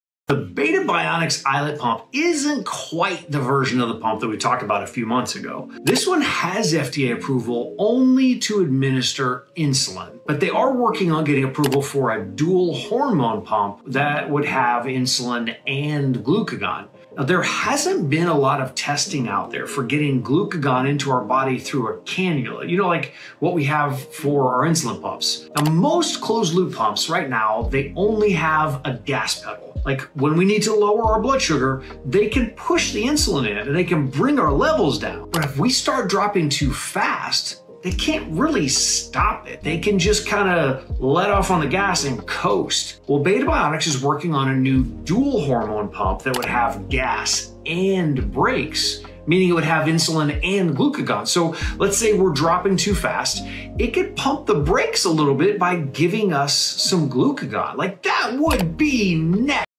Explaining Type 1 Diabetes // Part 306 The ilet pump from Beta Bionics is FDA approved for giving in
Explaining Type 1 Diabetes // Part 306 The ilet pump from Beta Bionics is FDA approved for giving insulin and they are working on getting approval for a dual hormone pump, with both insulin and glucagon! 🩸💉